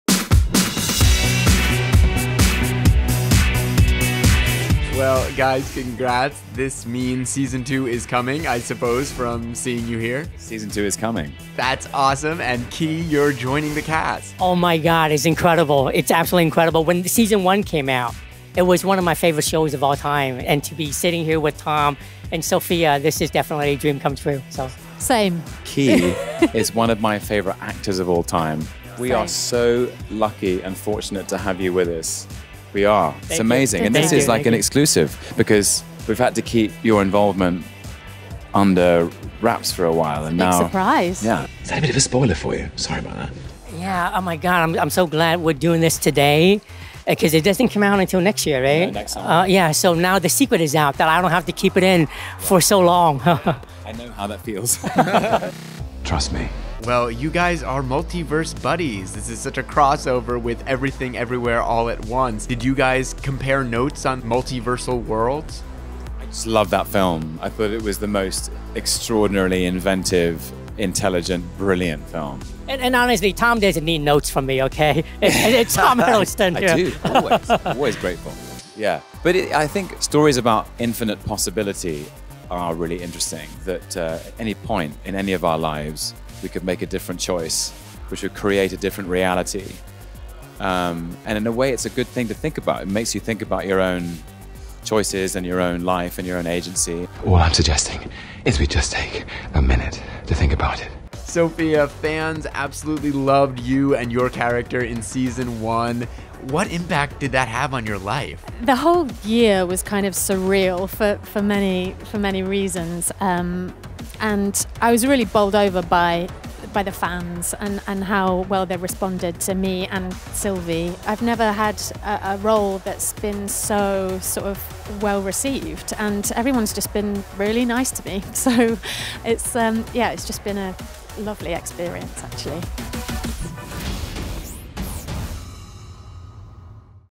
"Loki" Season 2 Cast interview | On the Scenes | IMDb
tomhiddleston and sophiadimartino, the actors of loki, welcome kehuyquan from Everything Everywhere All at Once to the cast for Loki season 2, during which they express their admiration for each other's work spanning multiple universes.